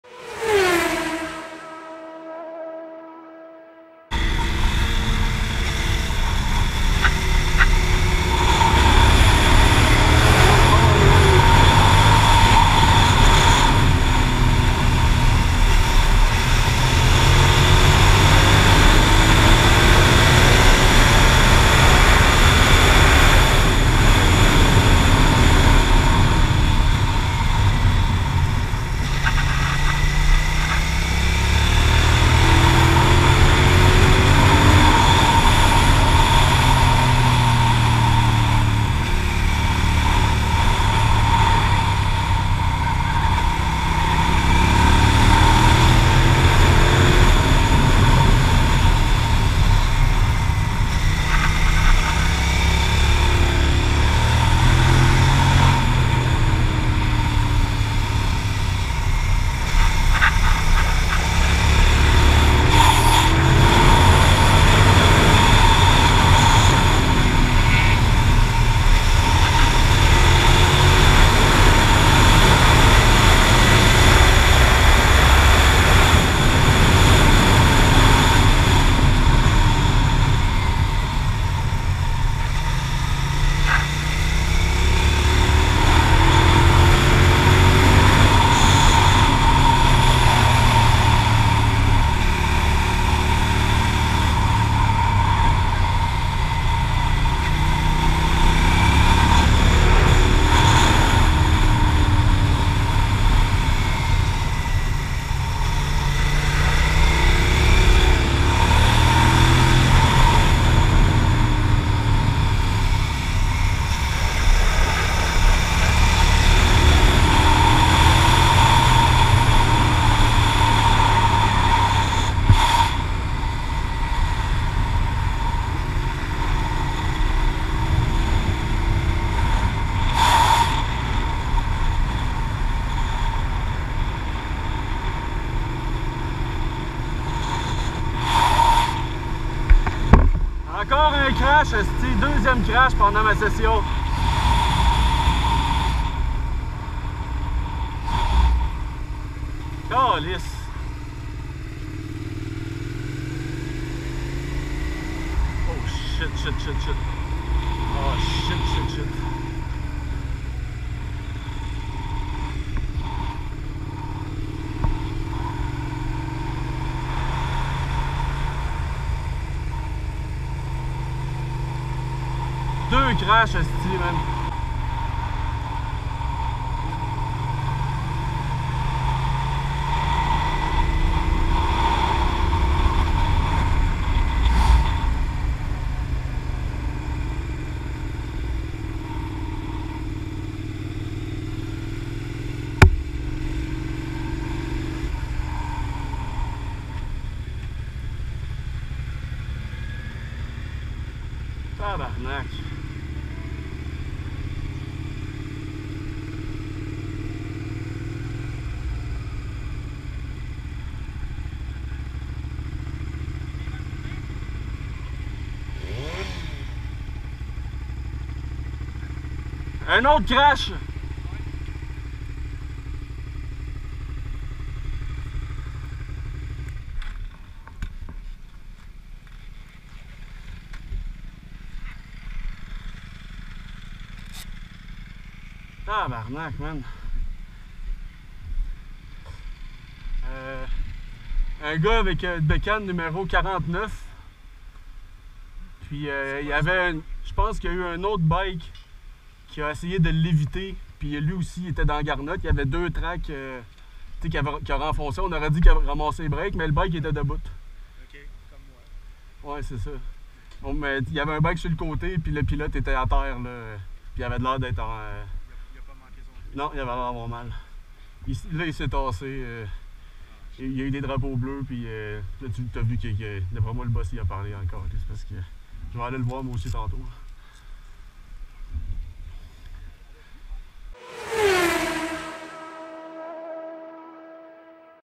BIG CRASH MECAGLISSE TRACKDAY MOTO
4 june 2023 green group 2nd crash of the day really sad day hopes the rider is fine